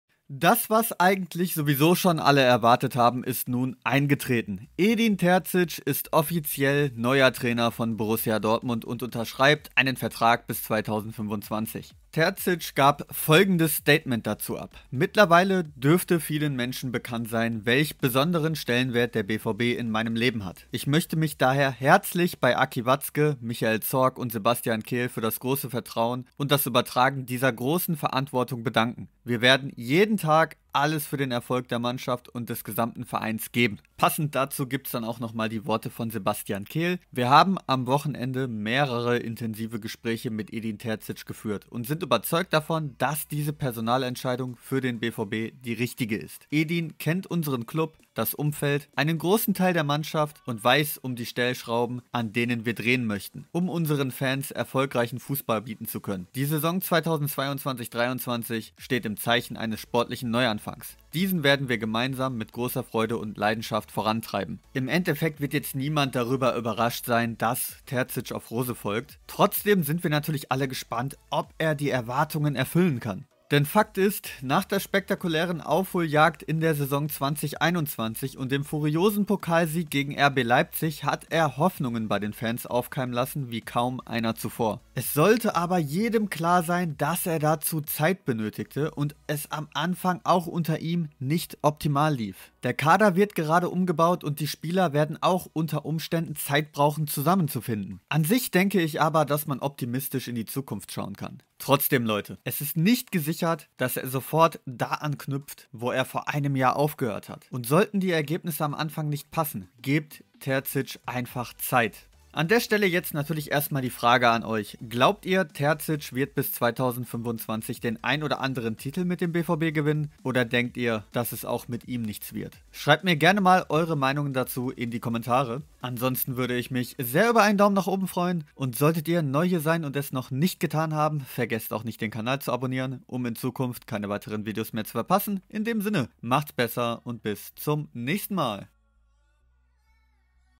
Offizell: Terzic übernimmt den BVB!
Edin Terzic ist nun Offiziell der Nachfolger von Marco Rose als Trainer bei Borussia Dortmund. Er unterschreibt einen Vertrag bis 2025.